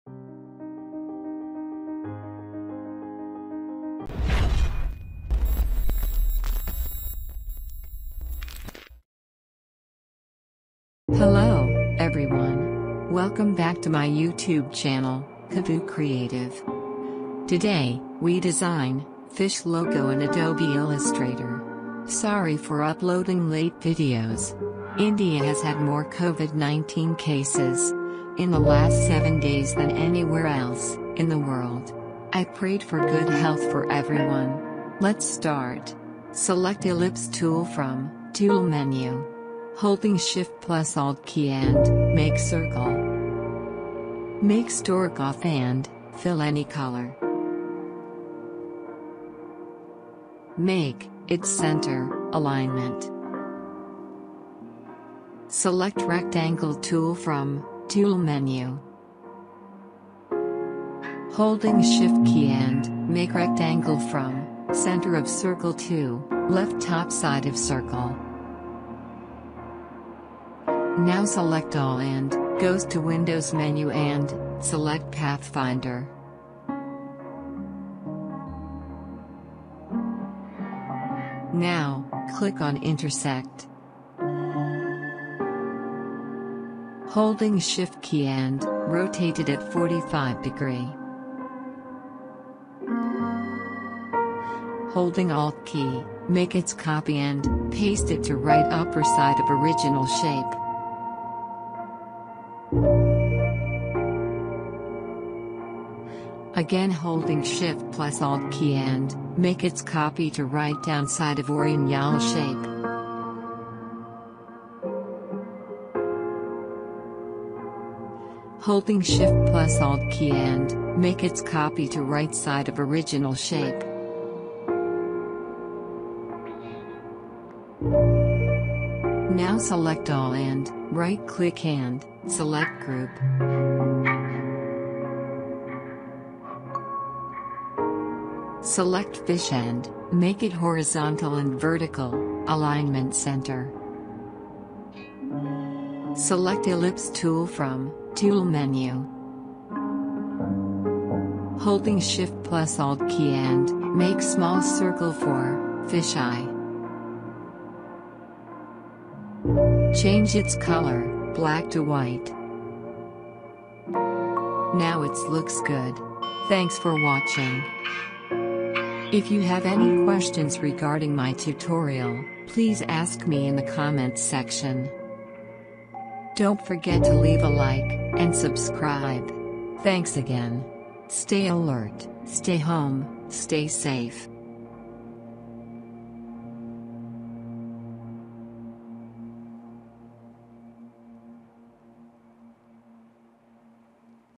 Logo Design Tutorial in Adobe Illustrator - Minimal Fish
In this video, I'll show you  How to Design Fish Logo in Adobe Illustrator.

This tutorial is for beginners.

This is my Social Profile Details, requesting you to let's meet on a social platforms for the latest updates.
FOLLOW ME (KavuCreative)